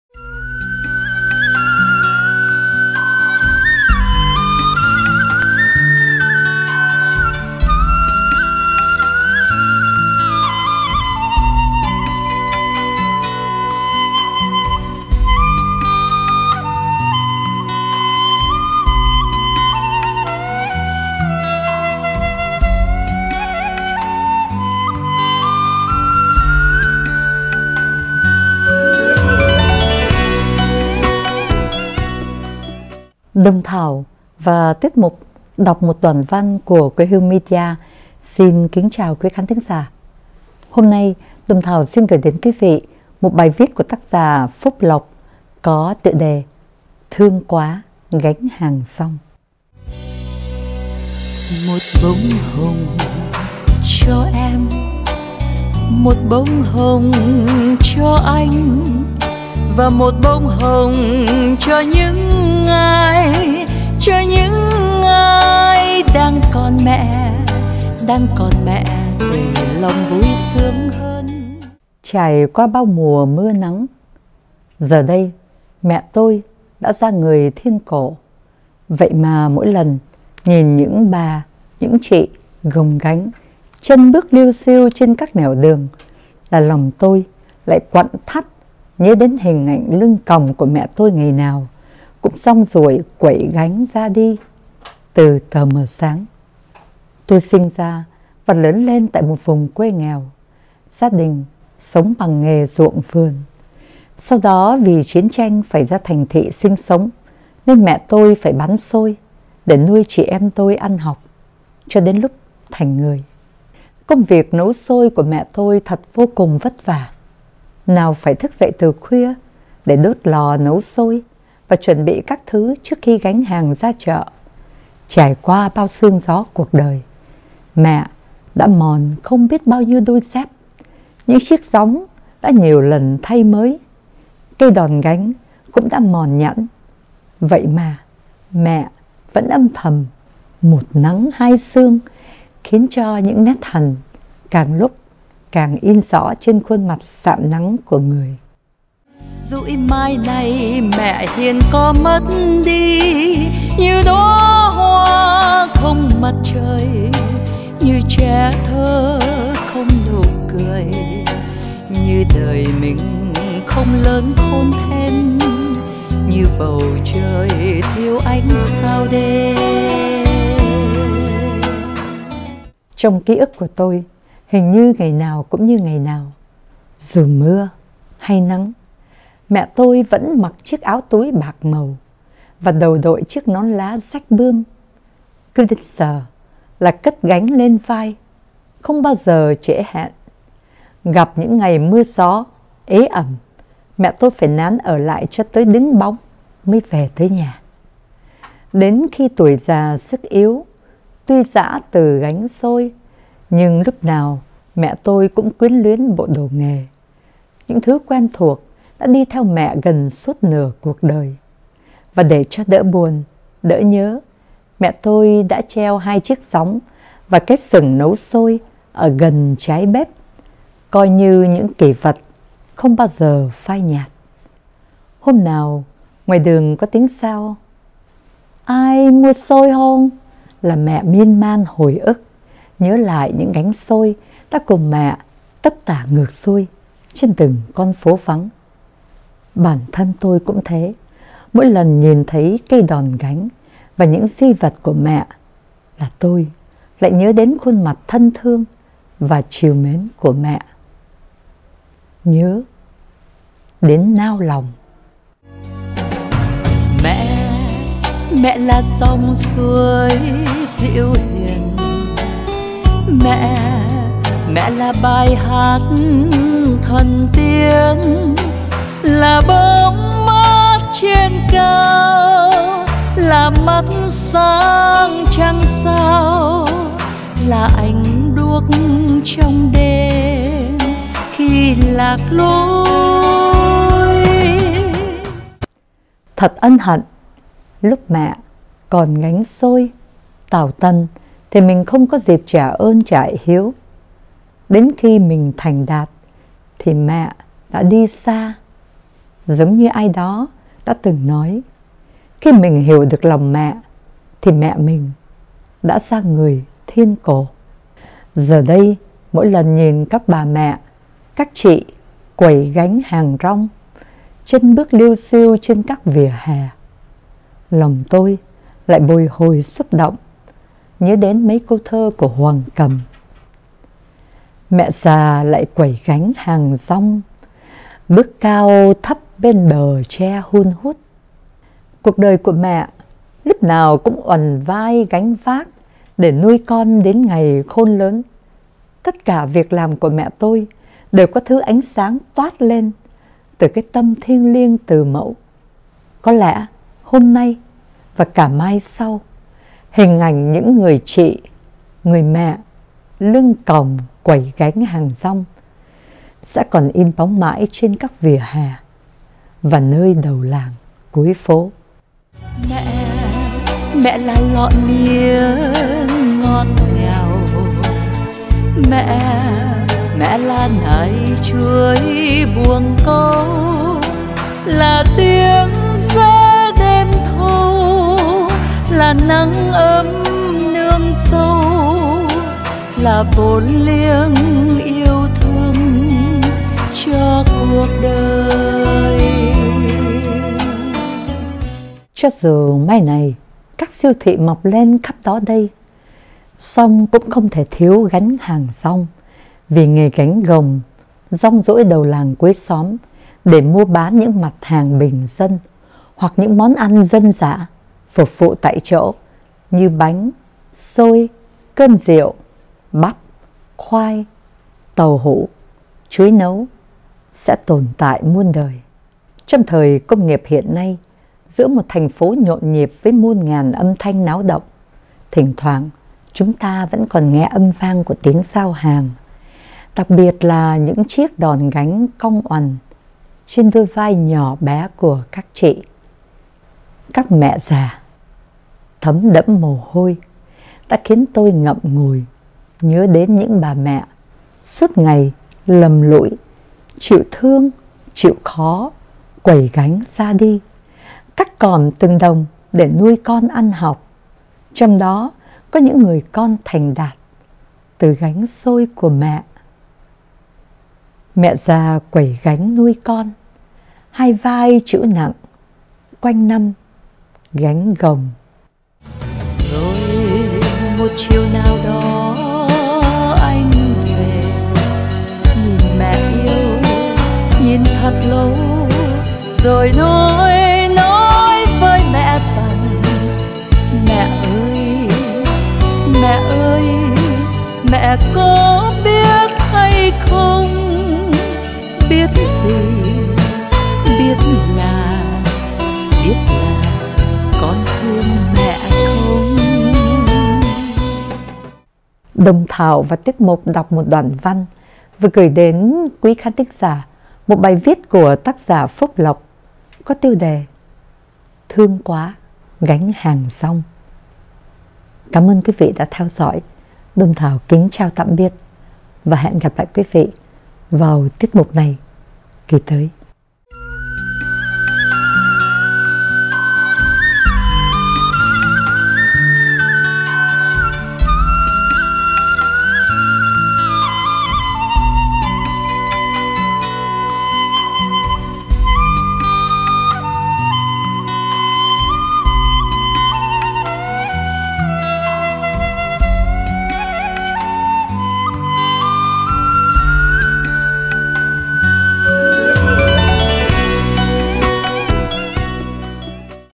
Đọc Một Đoản Văn - Thương quá gánh hàng rong- Phúc Lộc.
Đọc Một Đoản Văn- Đồng Thảo đọc. Quê Hương Media.
Thương quá gánh hàng rong, bài viết Phúc Lộc.